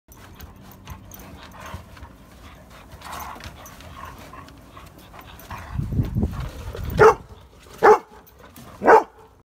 Mongo and Joshua and referee Diesel
2-year-old chocolate male Lab "Mongo" is available for adoption through Golden Gate Labrador Retriever Rescue